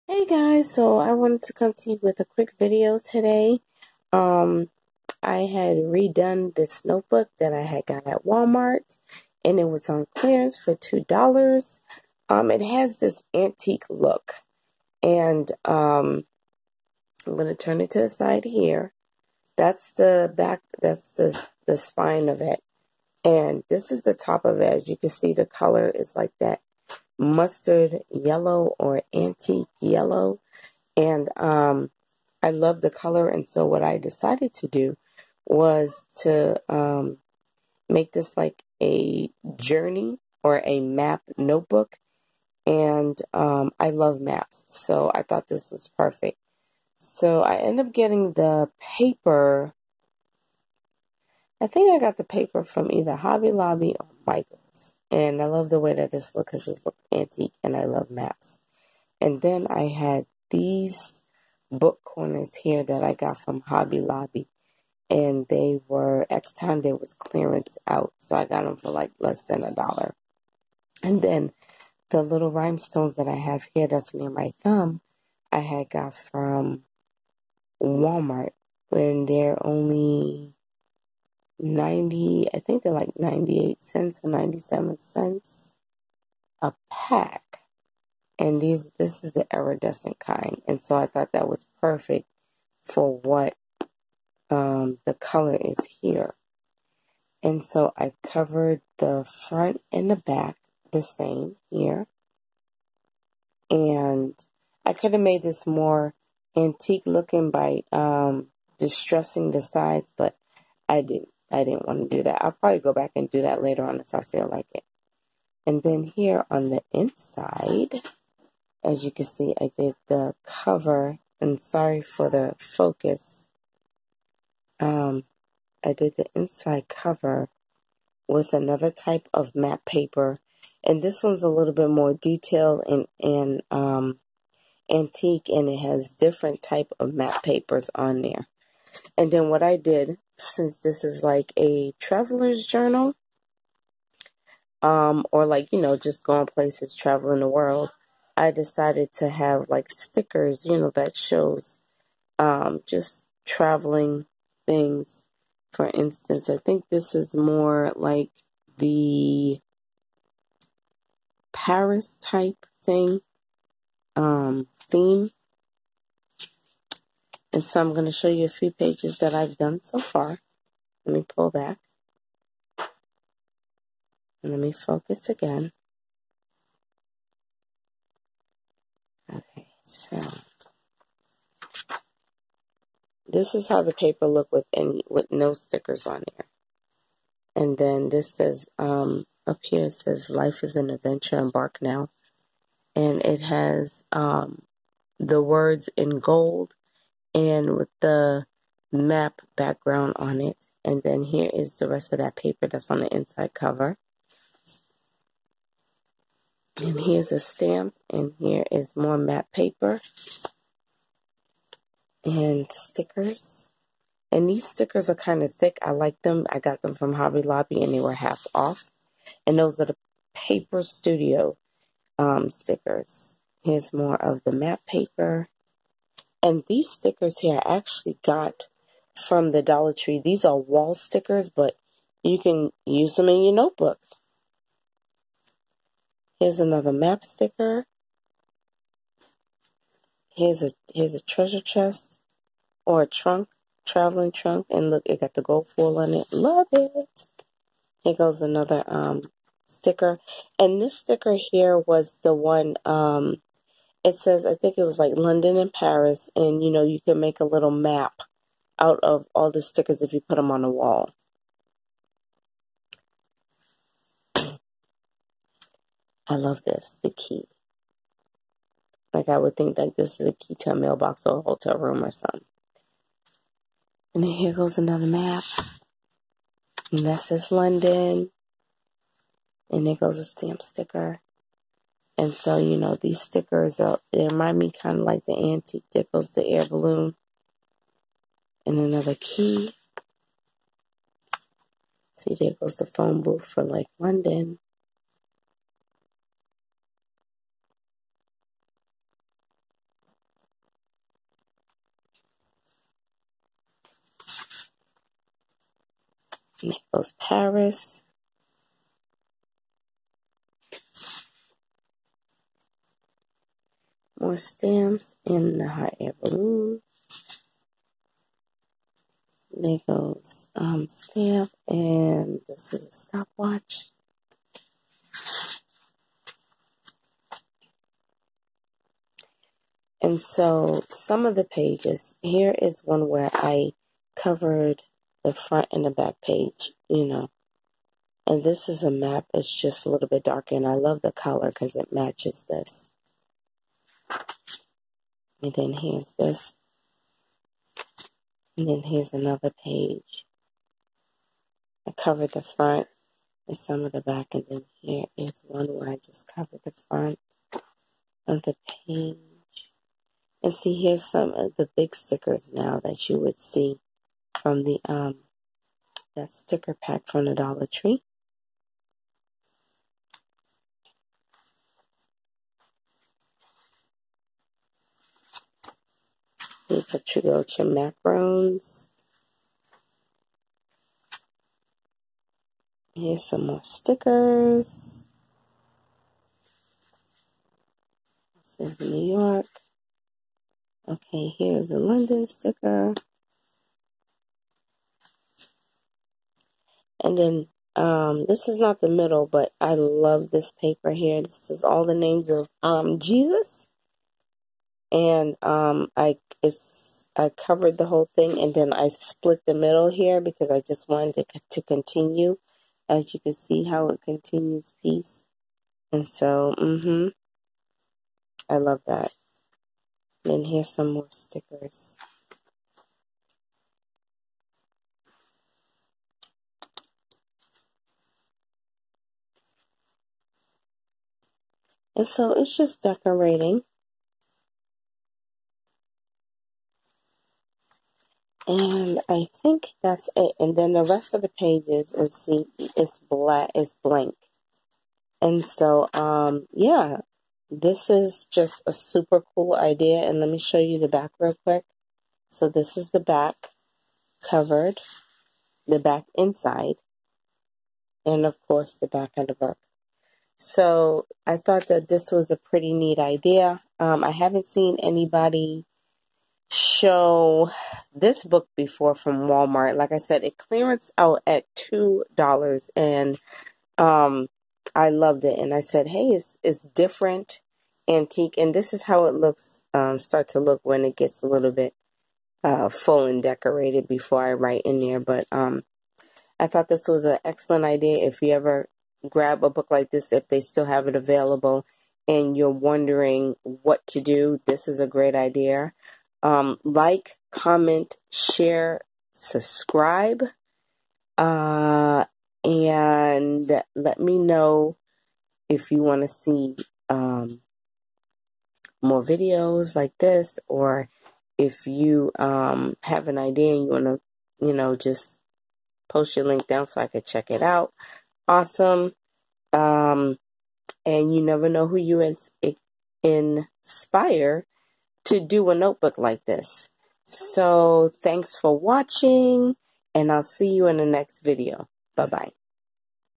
DIY World travel theme Notebook.
This is the first video with this craft notebook from Walmart. I customize it with world theme stickers from Hobby Lobby and the wall stickers from the Dollar tree along with other goodies. You will inspire someone to let their creativity flow!!! God bless you!